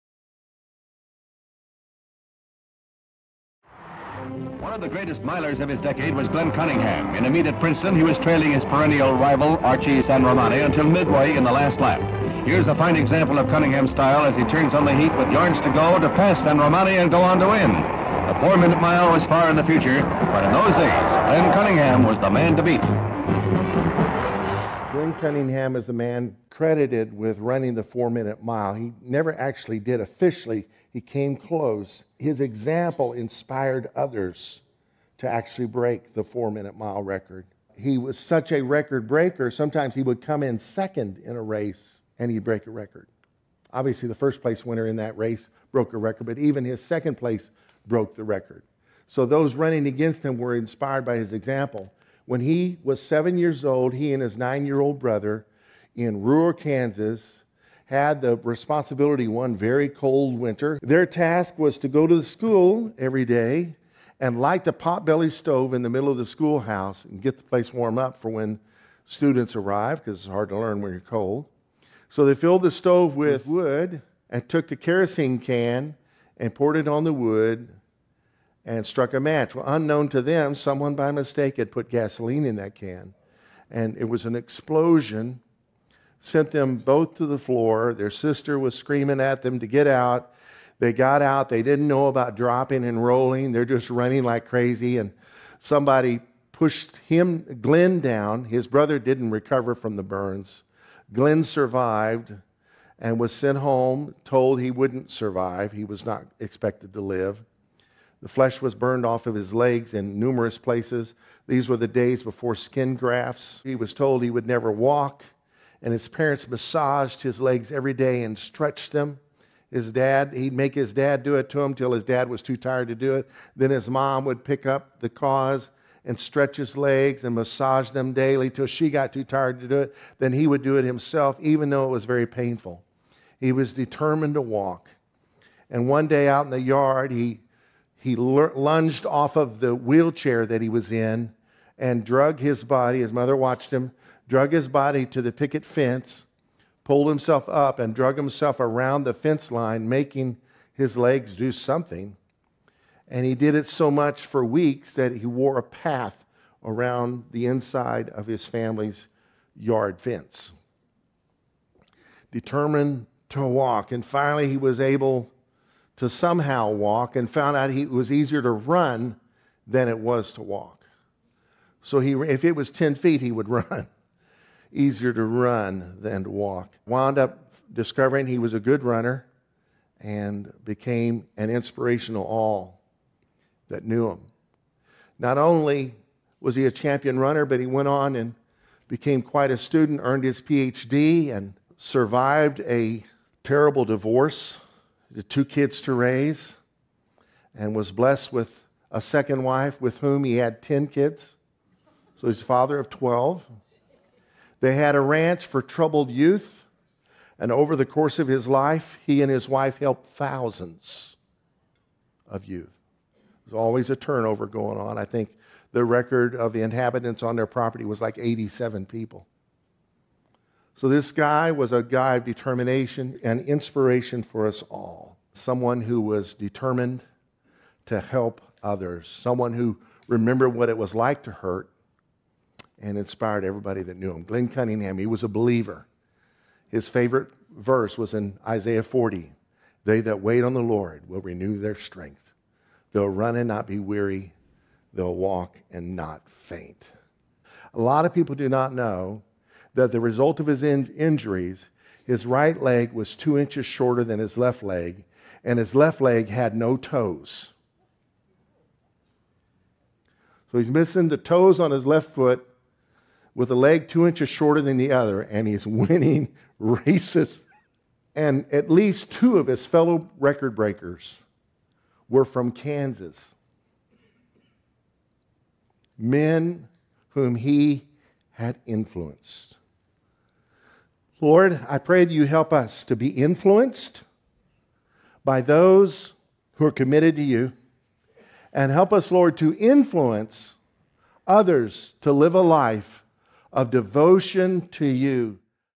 THE GLENN CUNNINGHAM STORY
Be inspired by the amazing story of Glenn Cunningham, two time Olympian and middle distance champion runner; who  overcame difficult circumstances and encouraged everyone around him throughout his amazing life.